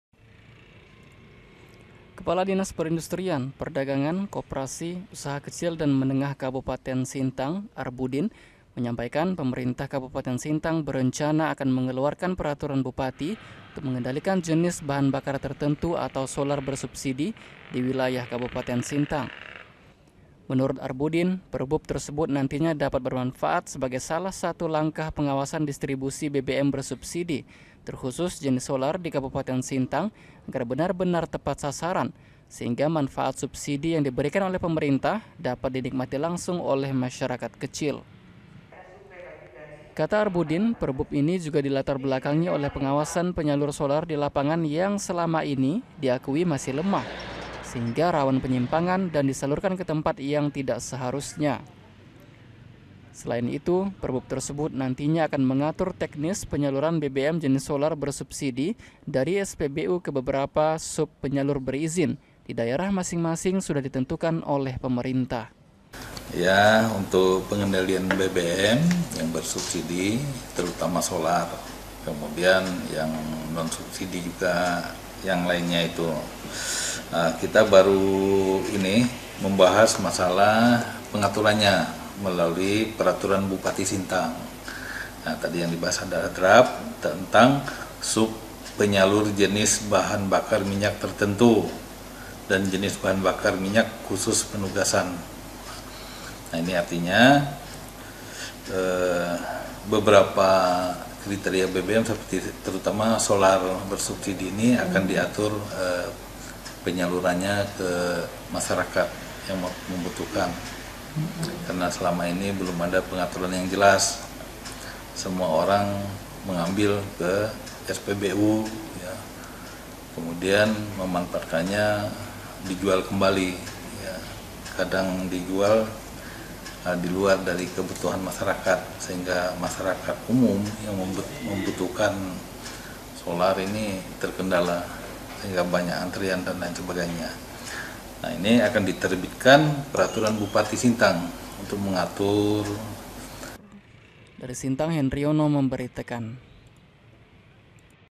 Susun Perbup Agar BBM Solar Bersubsidi Tepat Sasaran
Pemerintah Kabupaten Sintang sedang menyusun peraturan bupati, tentang pengendalian jenis bahan bakar tertentu atau solar bersubsidi, agar tepat sasaran, dan manfaat subsidi yang diberikan oleh pemerintah, benar –benar dapat dinikmati langsung oleh masyarakat kecil